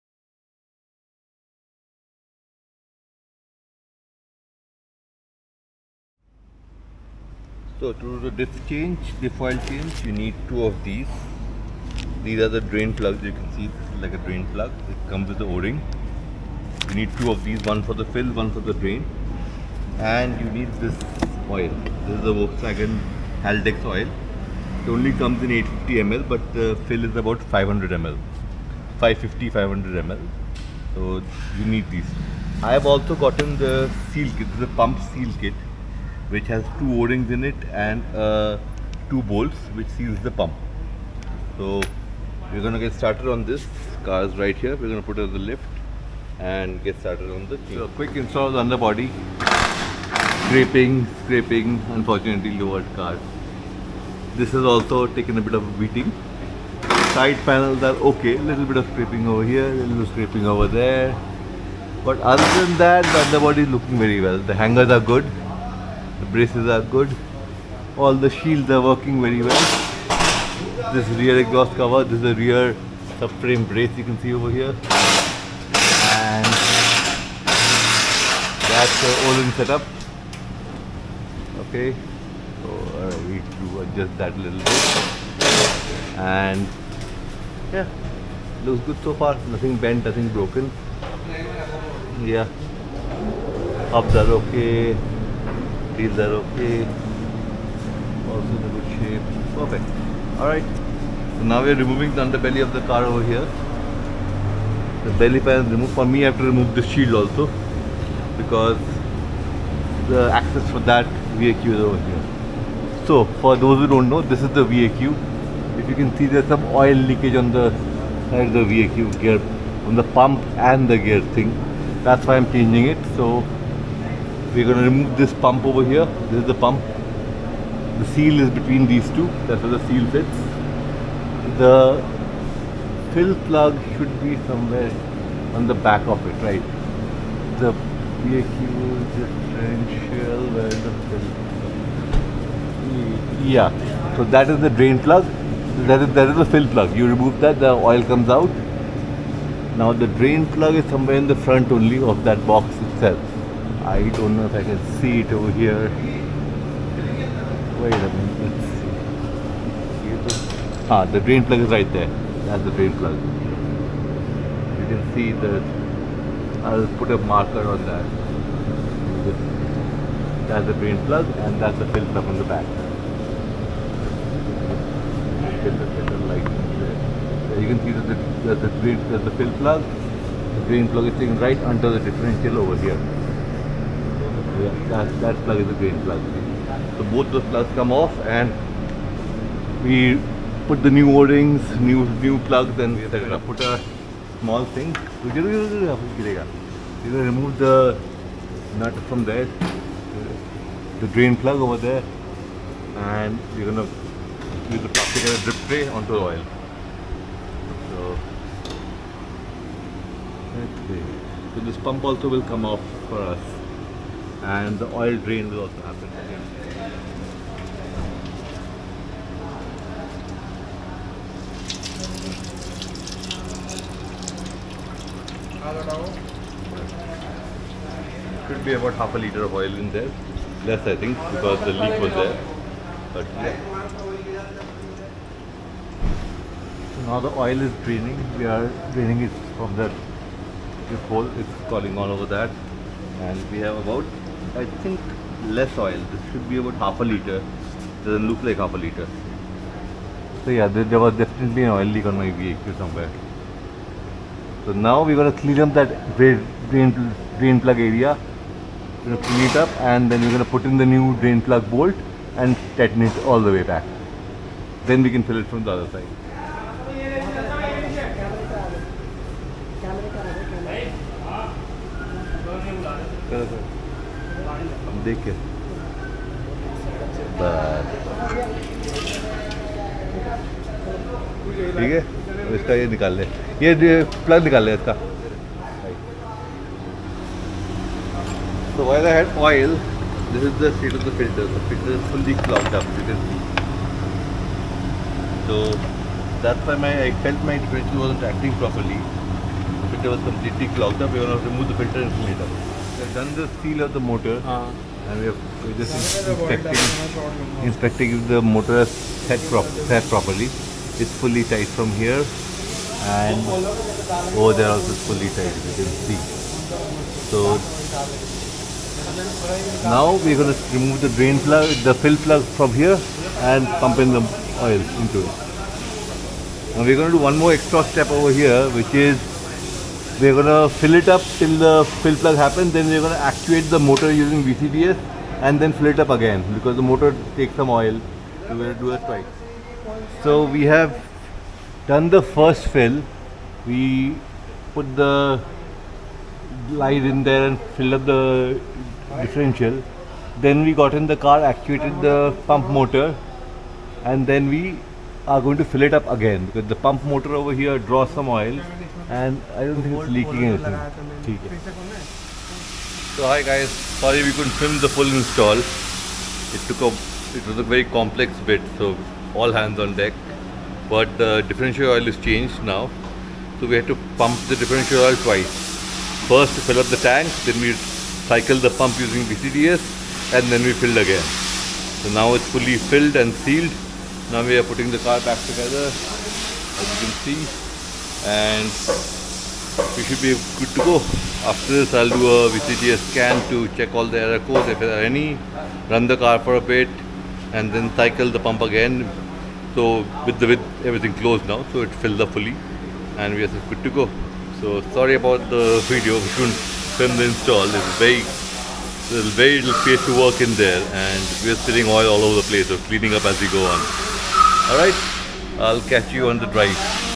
Octavia RS245 VAQ Diff Service - The Process
Here is the video of the process I followed for the VAQ oil change.
Parts needed -
VAQ Front Haldex Oil G060175A2 X 1
Drain Plugs WHT007168 X 2
VAQ Haldex Pump Repair Set 0CQ598305 X 1

How will you know if the oil is full - simple! Fill till it overflows out, wait for excess to drain out, and that's it.
Once you do the first oil fill, you need to go into VCDS - Diff Lock and run the reset adaptation, the pump cycle adaptation and then add some more oil before sealing off the drain plug.